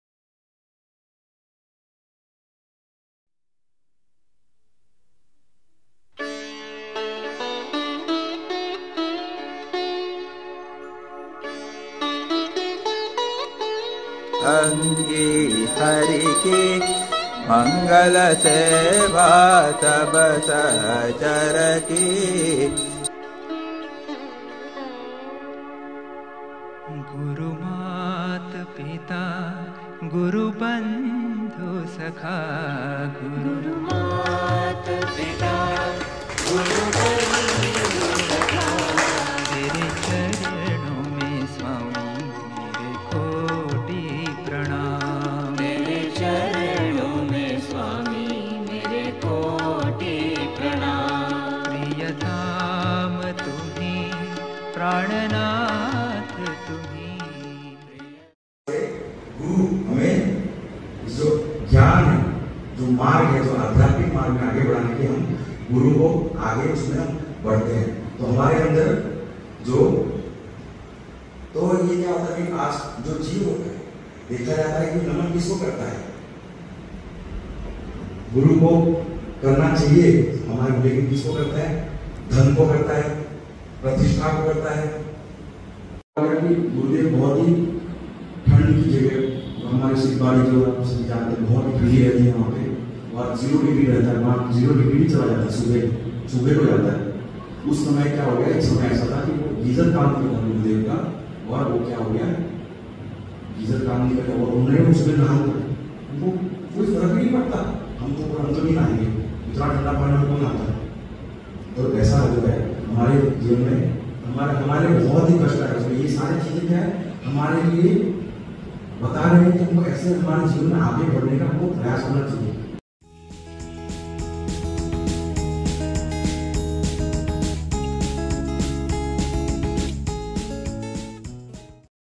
Glimpses of Satsang by Br. Pushkar Chaitanya।। Divya Satsang Mala Day 2।।
Day 2 of Divya Satsang Mala organised on the occasion of 15th Vardhapan Day of Chinmaya Aradhana Ashram Ponda Goa. Talk By Br. Pushkar Chaitanya on Tasmai Shri Gurave Namah.
Venue Chinmaya Aradhana Ashram Ponda Goa.